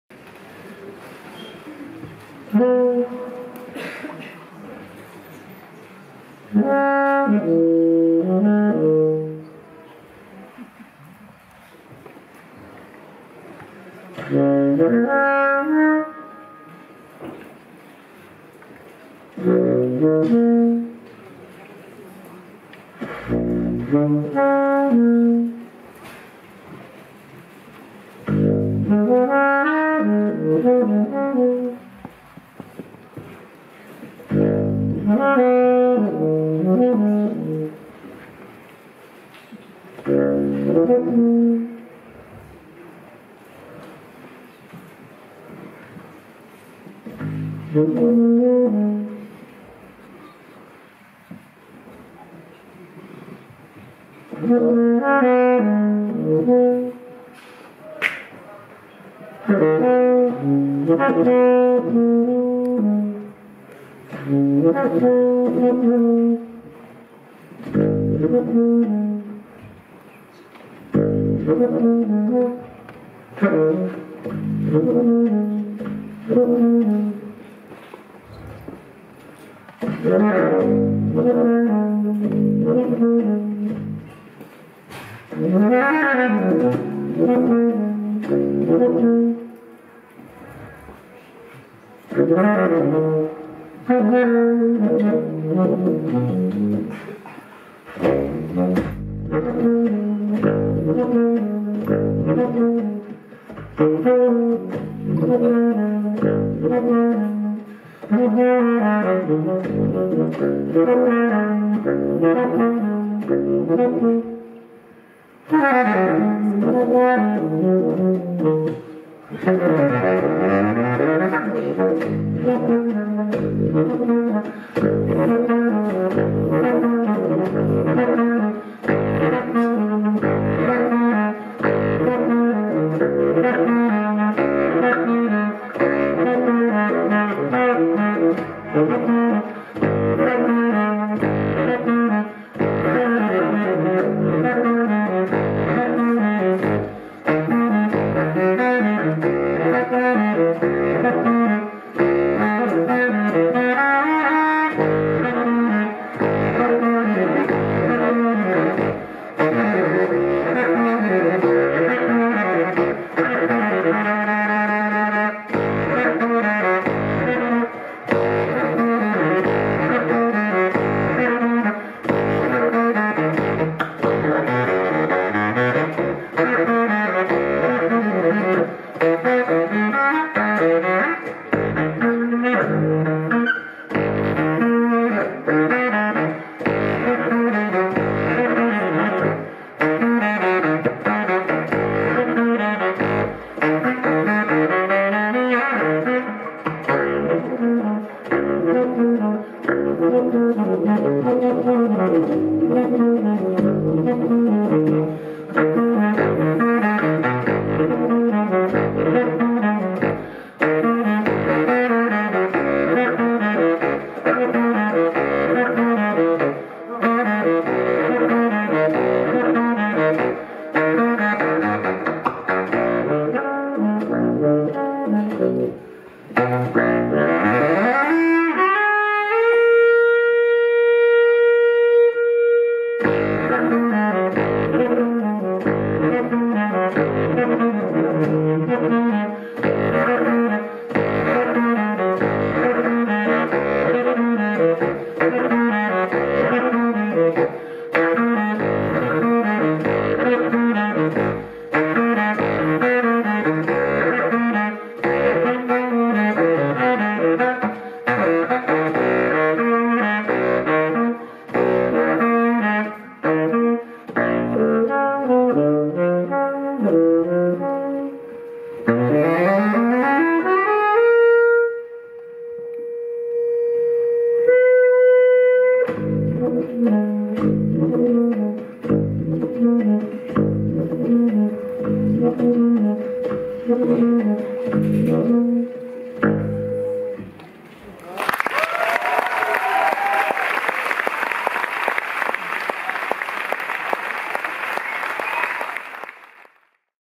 Fred Gastard Solo Live #2 au Café de la Danse le 24 Septembre 2019
Fred Gastard Solo (EXTRAIT)
 Live #2 au Café de la Danse le 24 Septembre 2019
en première partie de Brigitte Fontaine